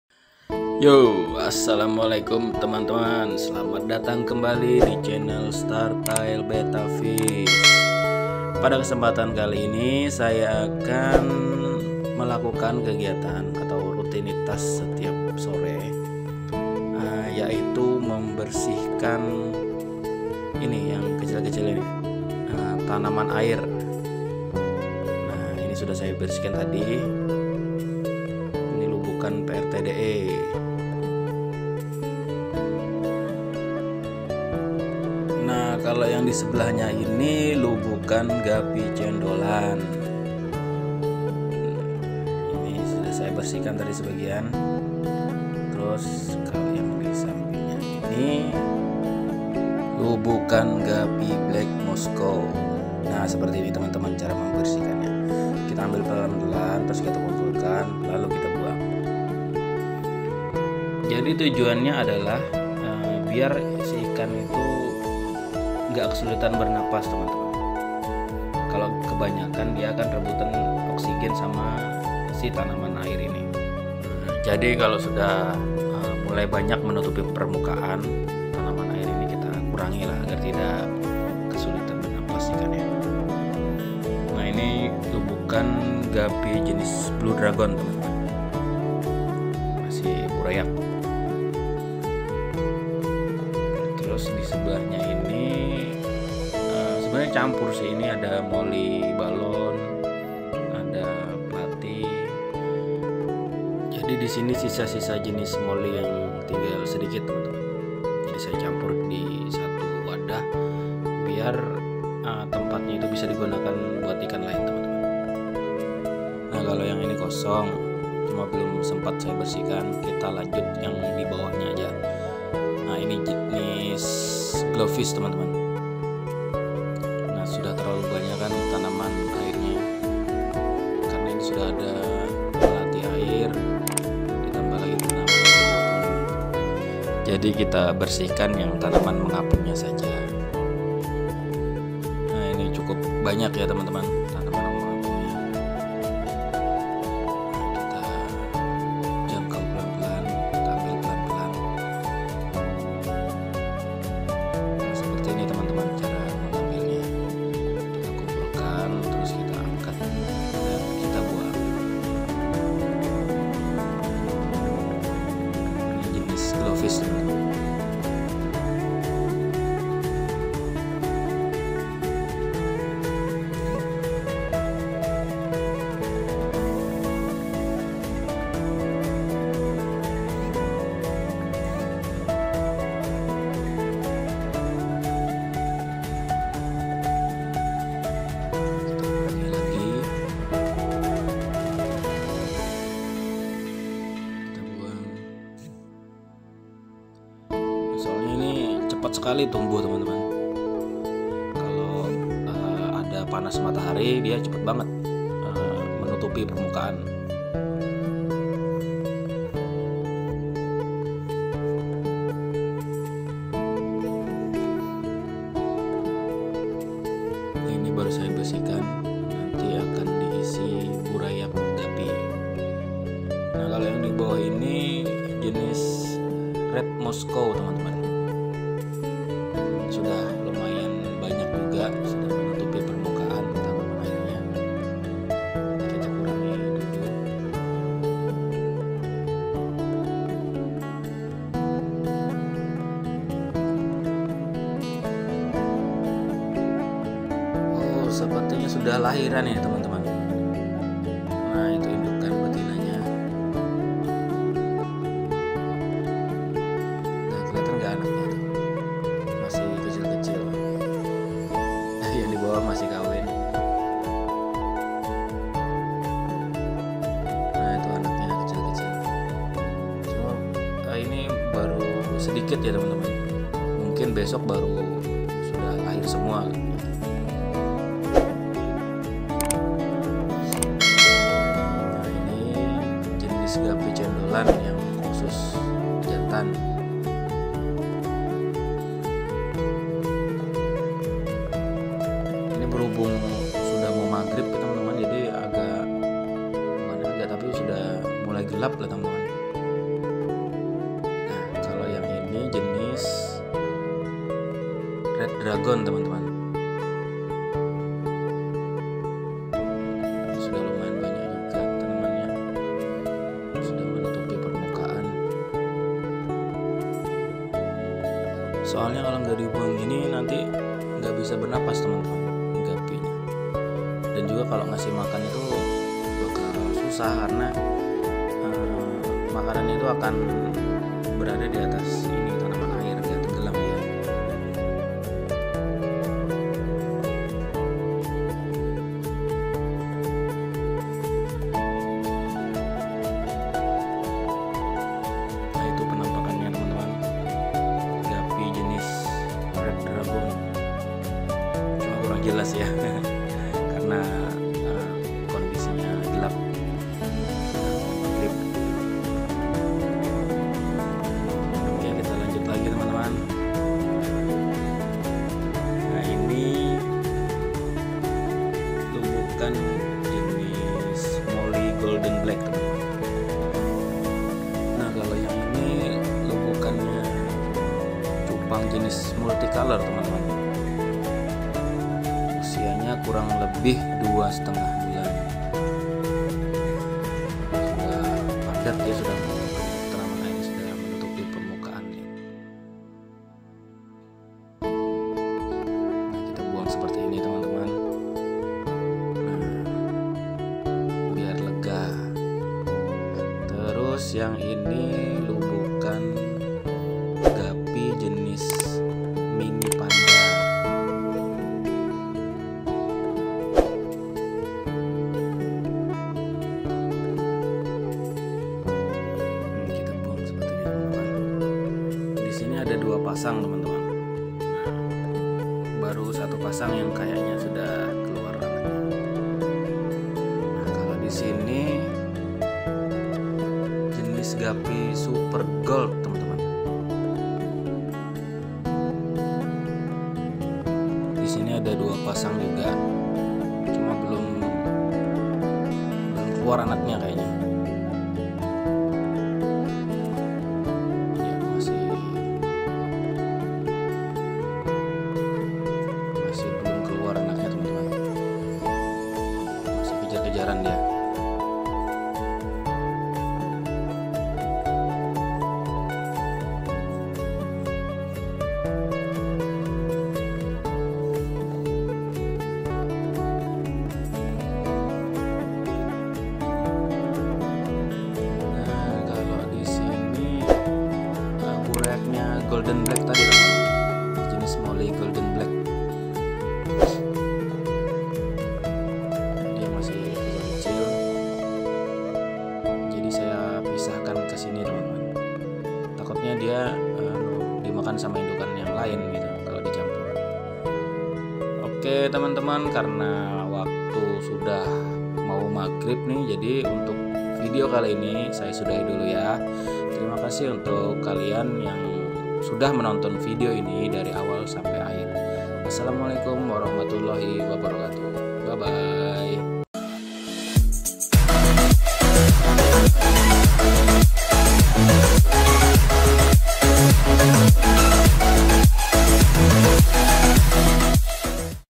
GAWAT!! Ini penyebab ikan sering mati!
Rutinitas sore hari membersihkan tanaman air yang sudah penuh sesak di Farm Startail Bettafish.

Tumbuhan air memang sangat bermanfaat bagi kenyamanan tempat tinggal ikan di lubukan, selain untuk menaungi dari panas matahari, tumbuhan air juga berfungsi untuk habitat biota air yang lebih kecil sebagai pakan alami burayak ikan.